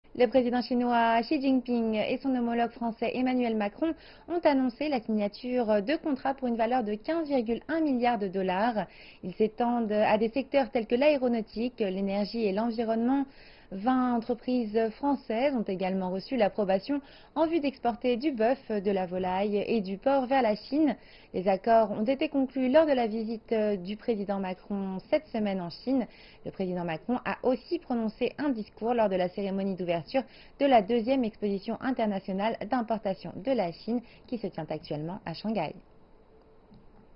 Plus de 15 milliards de dollars de contrats signés entre la Chine et la France
Le président chinois Xi Jinping et son homologue français Emmanuel Macron ont annoncé la signature de contrats pour une valeur de 15,1 milliards de dollars.  Ils s'étendent à des secteurs tels que l'aéronautique, l'énergie et l'environnement. Vingt entreprises françaises ont également reçu l'approbation en vue d'exporter du boeuf, de la volaille et du porc vers la Chine. Les accords ont été conclus lors de la visite de Macron cette semaine en Chine. Le président Macron a aussi prononcé un discours lors de la cérémonie d'ouverture de la 2e Exposition internationale d'importation de la Chine qui se tient à Shanghai.